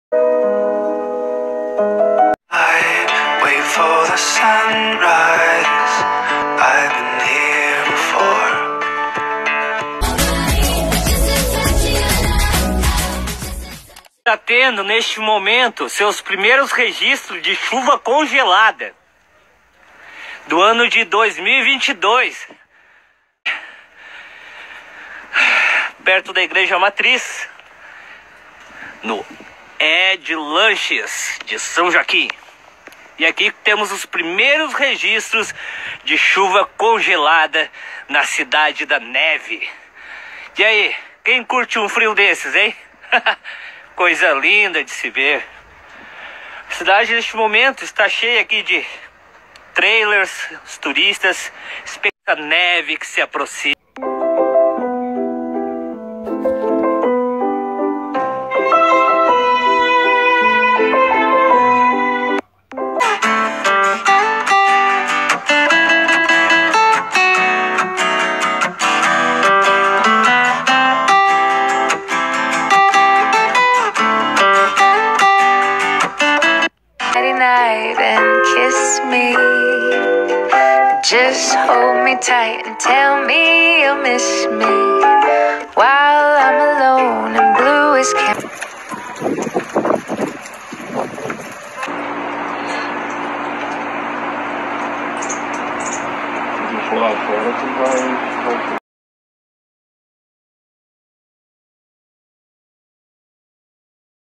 🔵Primera NEVADA en Santa Catarina del 2022 ❄ Frio en Joinville |Venezolanos en Brasil
La paz de Dios sea contigo!